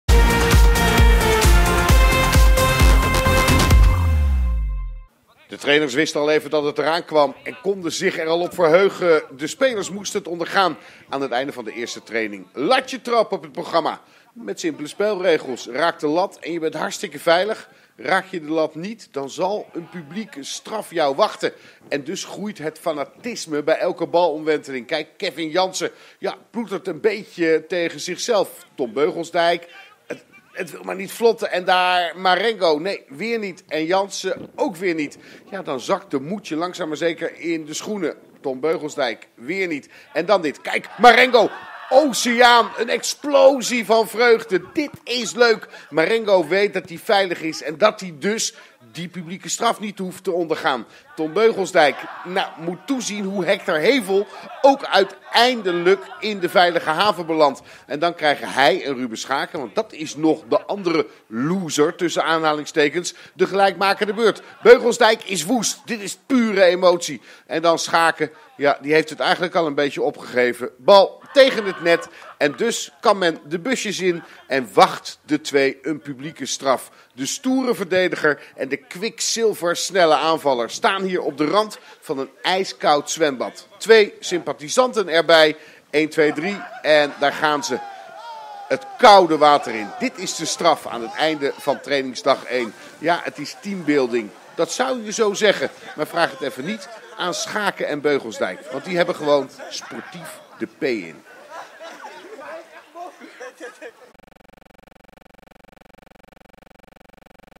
Schaken en Beugelsdijk verliezen latje trap en krijgen letterlijk een koude douche
Ruben Schaken en Tom Beugelsdijk verloren tijdens de eerste dag van het trainingskamp een spelletje latje trap en dat kwam hun, samen met Edouard Duplan en Segun Owobowale letterlijk op een koude douche te staan.